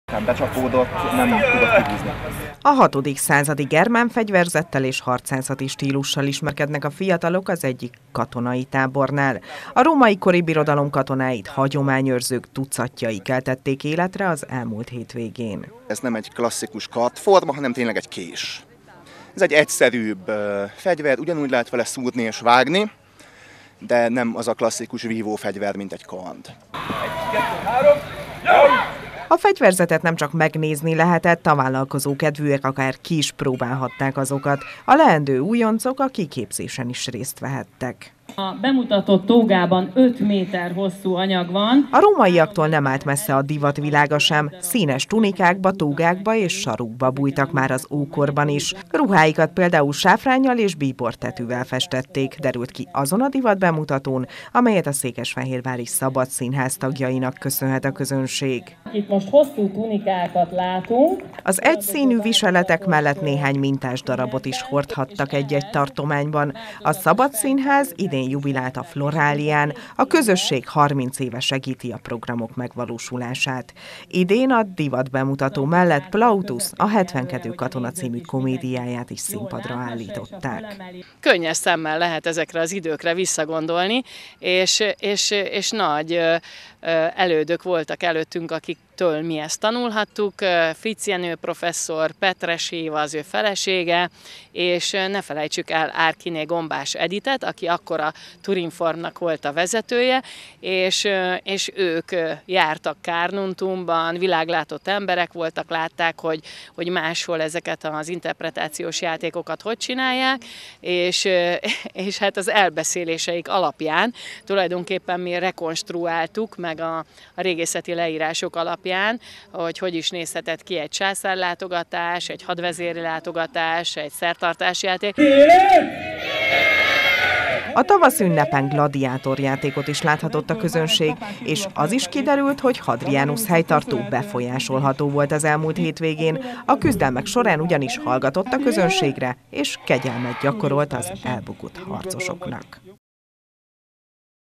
FLORALIA GORSIUMBAN
Történelmi időutazásban lehetett része annak, aki a hétvégén Gorsiumba látogatott. A római birodalom kelt életre az ókori romvárosban, ahol Hadrianus helytartó tett látogatást.